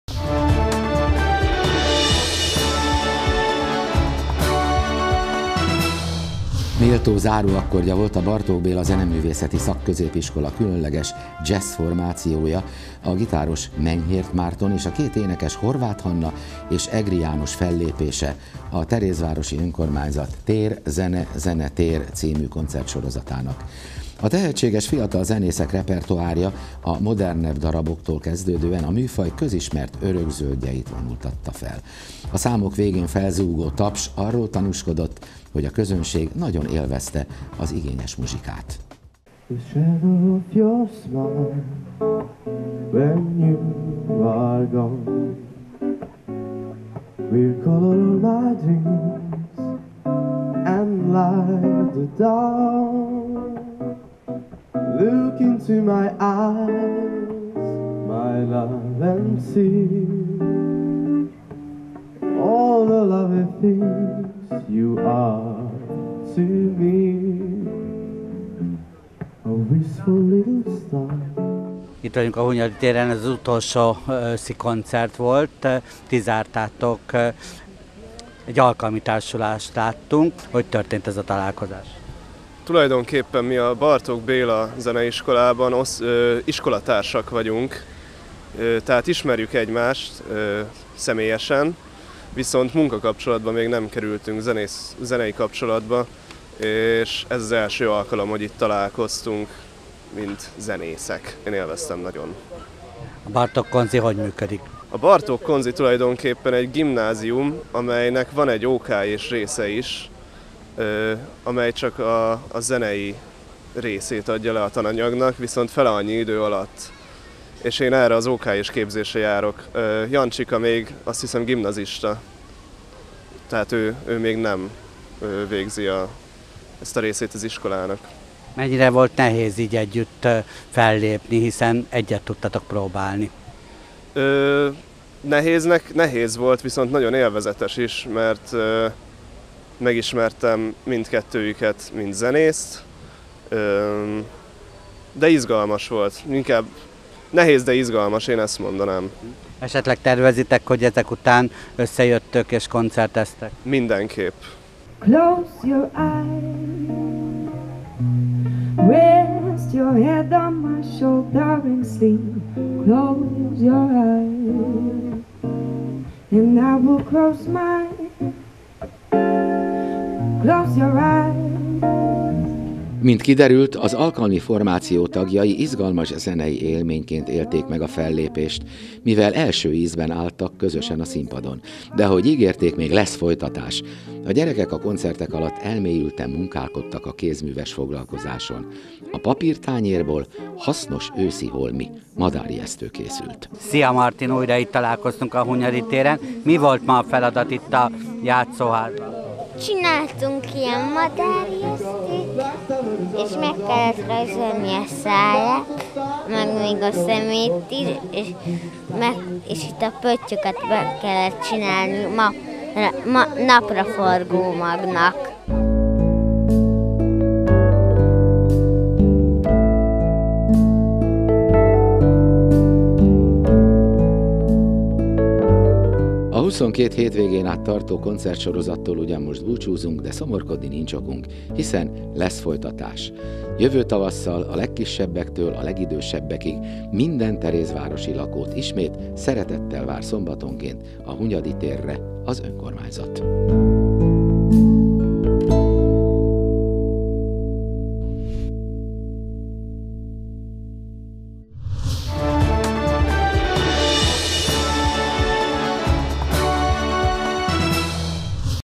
Ez Történt A Héten 2015.09.21. VII. rész. hatoscsatorna
DZSESSZKONCERTTEL BÚCSÚZOTT A HUNYADI TÉR

felelős szerkesztő: Szalai Zsolt
hírolvasó/narrátor: Dombóvári Gábor
szerkesztő-vágó: Gerstenbrein Zsuzsi
főszerkesztő: Paor Lilla